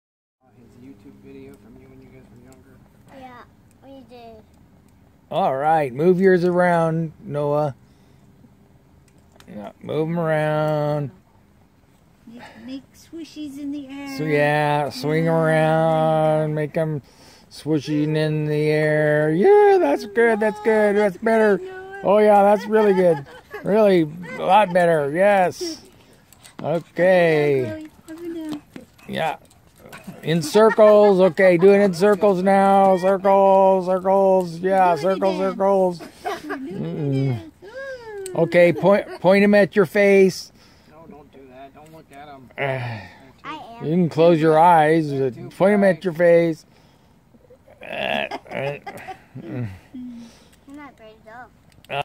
Grandkids with Finger Lights
Grandchildren Chloe and Noah just arrived for a visit this evening. I gave them finger lights to play in the dark.